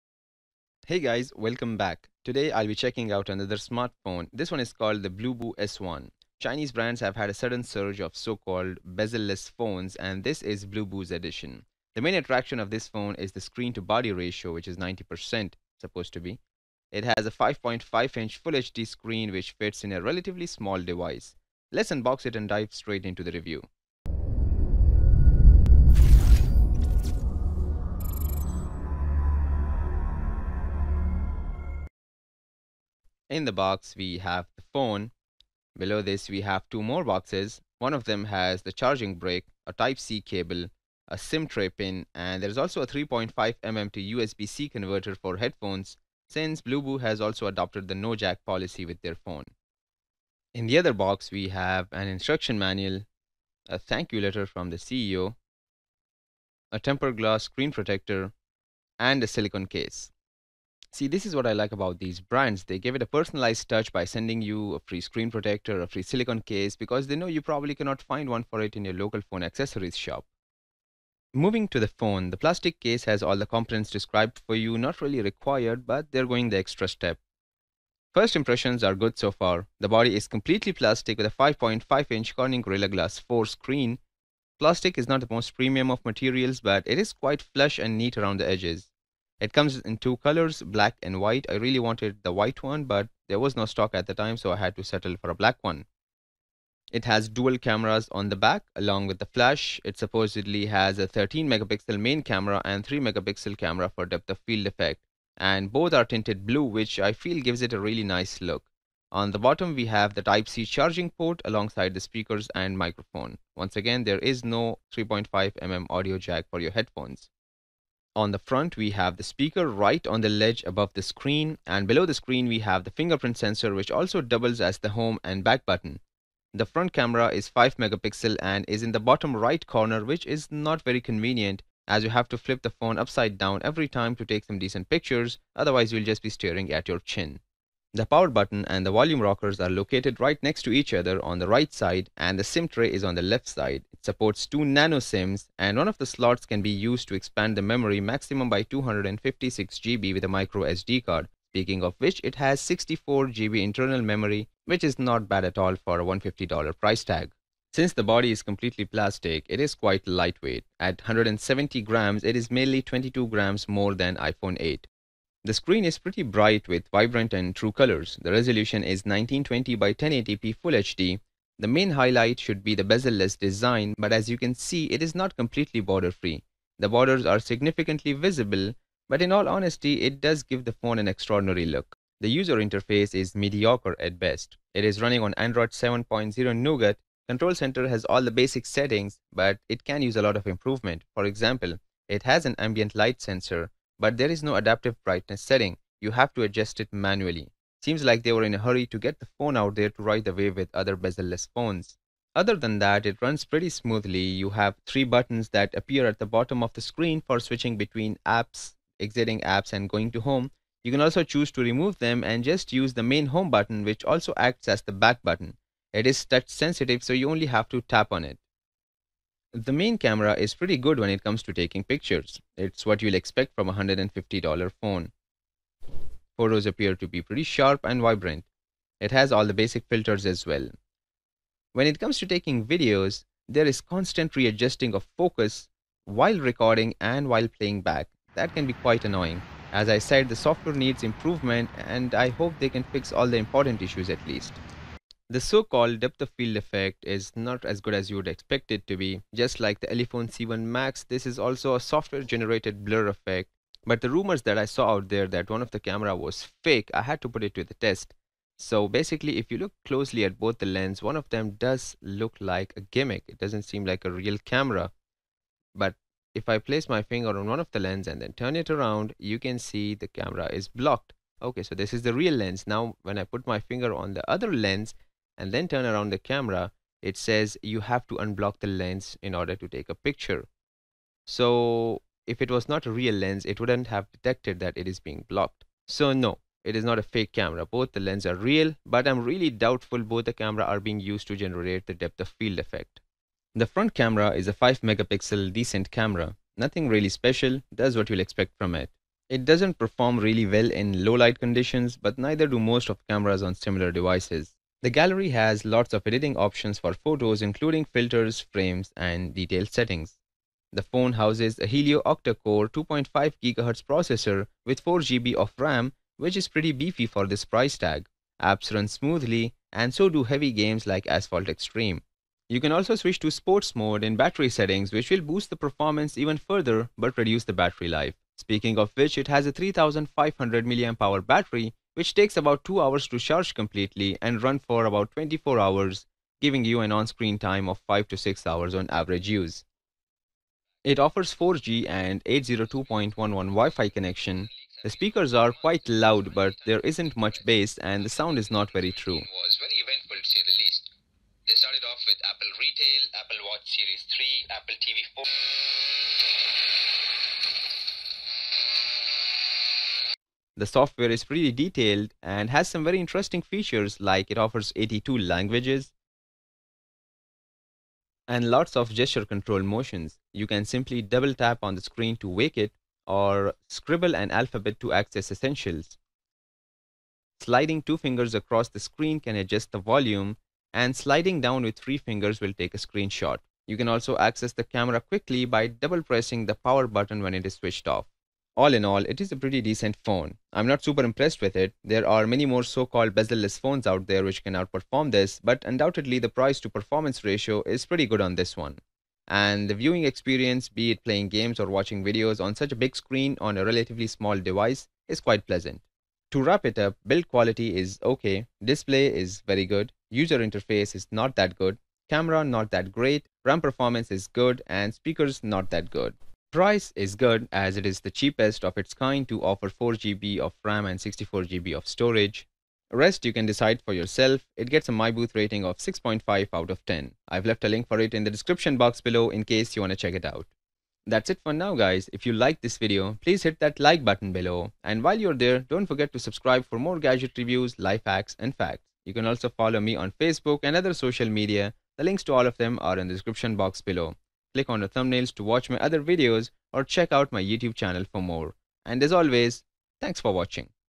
Cheapest Bezel-less Bluboo S1 Review | Is the 2nd Camera FAKE?
Hands on the cheapest bezel-less phone with dual camera. Rumor has it one of the camera is fake. Well, I had to test it.

G+: +Miibooth